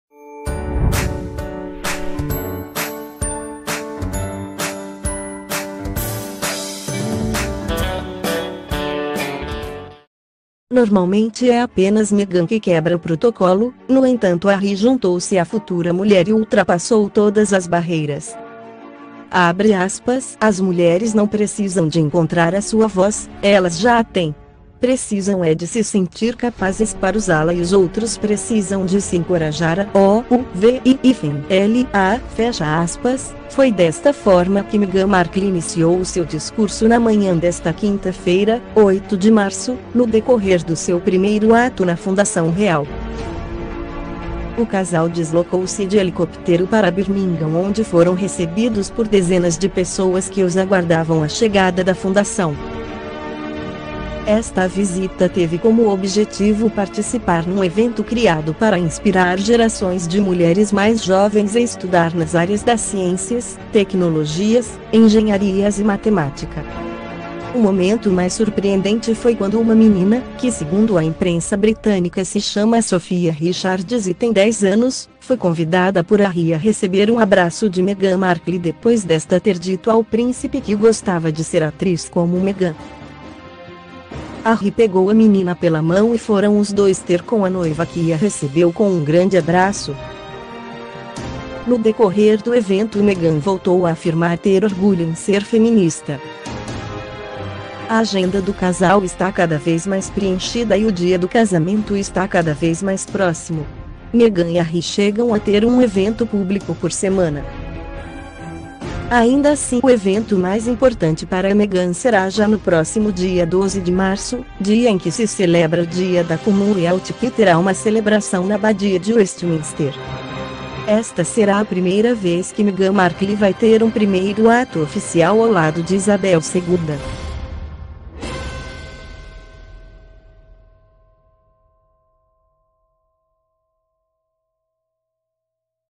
Harry e Meghan - Quebram protocolo prol de sonho de criança
Normalmente é apenas Meghan que quebra o protocolo, no entanto Harry juntou-se à futura mulher e ultrapassou todas as barreiras.
[time]«As mulheres não precisam de encontrar a sua voz, elas já a têm. Precisam é de se sentir capazes para usá-la e os outros precisam de se encorajar a ouvi-la», foi desta forma que Meghan Markle iniciou o seu discurso na manhã desta quinta-feira, 8 de março, no decorrer do seu primeiro ato na Fundação Real.